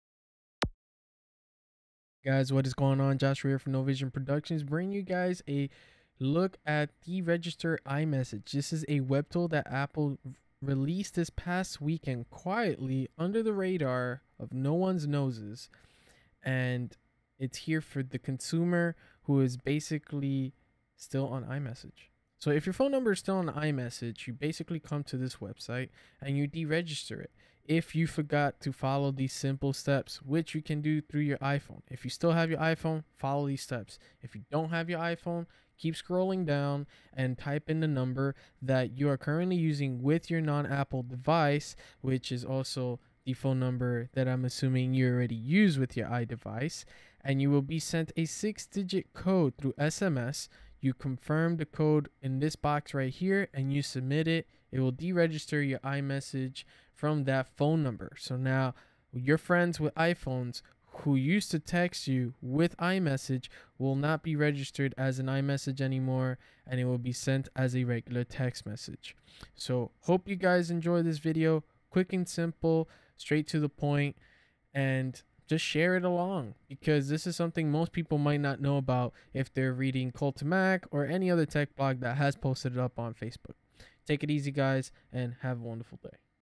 Deregister iMessage in 2 Easy Steps
Hey Guys,
In this video we look at a web tool that Apple released quietly on November 9th, 2014 to deregister iMessage from your current phone number.

This tool is only intended for consumers who previously used an iPhone and forgot to turn off their iMessage before switching to another mobile device.